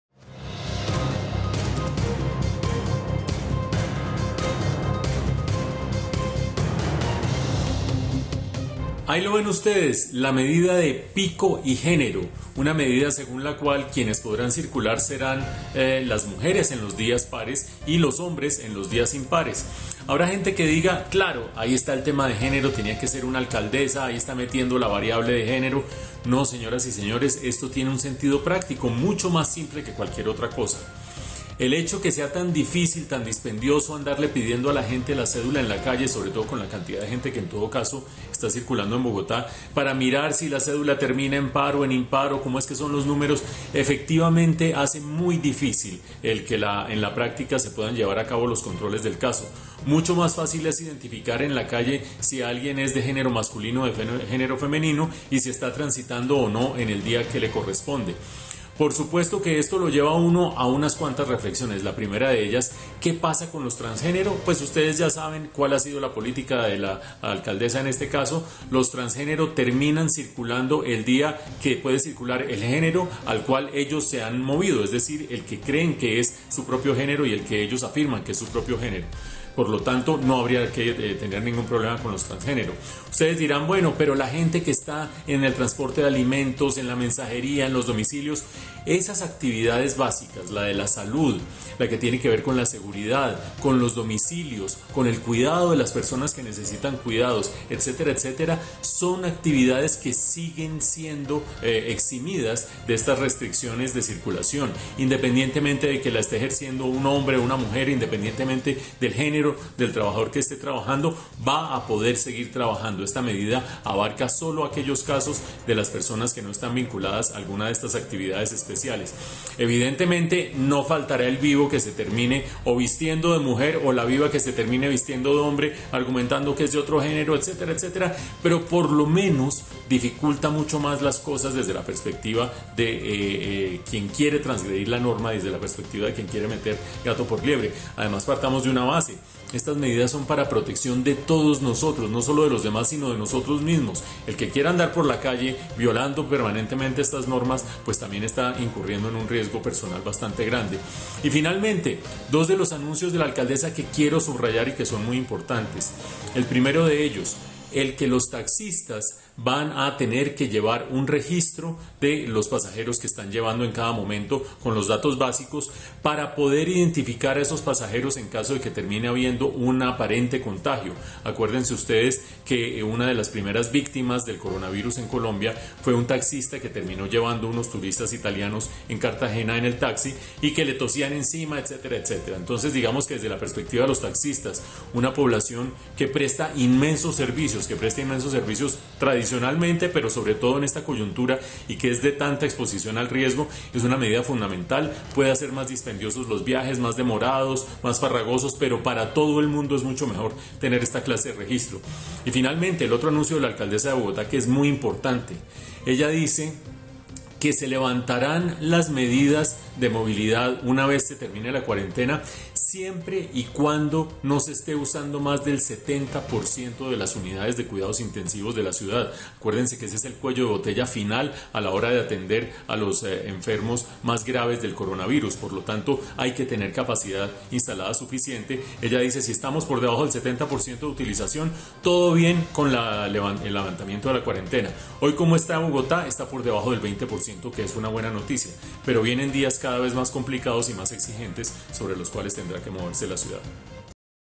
Red+ | ¡’Pico y género’ en Bogotá!
¿Tiene sentido la iniciativa de la alcaldesa mayor de Bogotá, Claudia López, de restringir la salida a la calle por género?